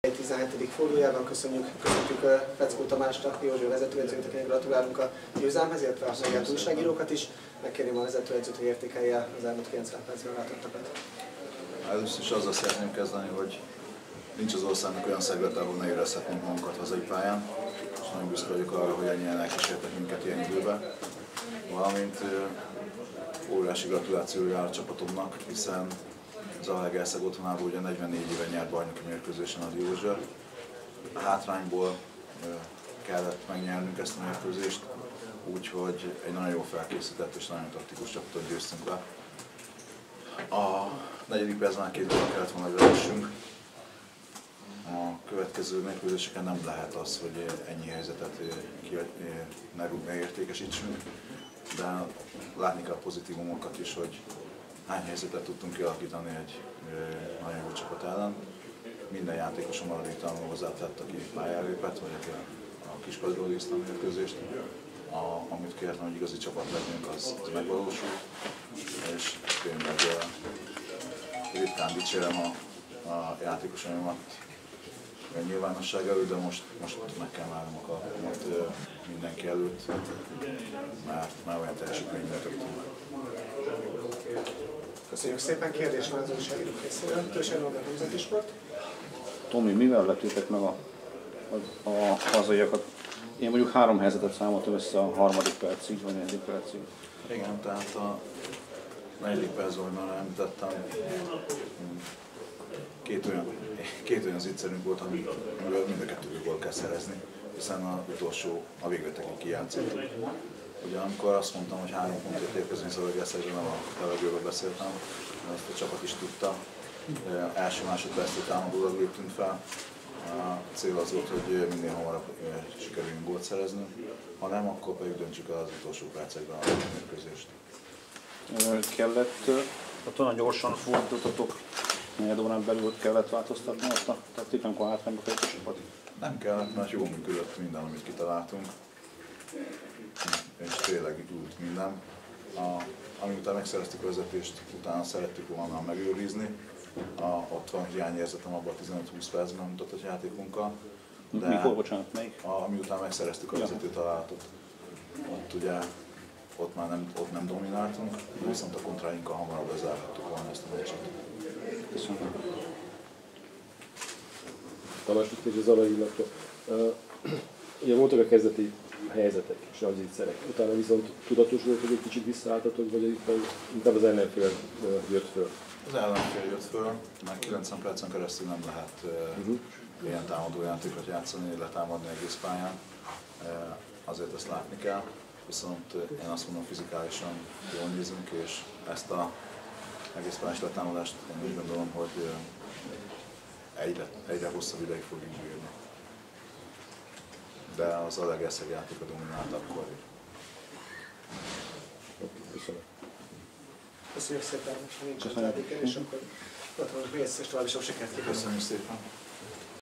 ZTE vs. DVTK 19/20 - Feczkó Tamás értékelése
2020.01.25., ZTE FC - DVTK 1-3.
Feczkó Tamás, a Diósgyőr vezetőedzője válaszolt a sajtó munkatársainak a mérkőzés után.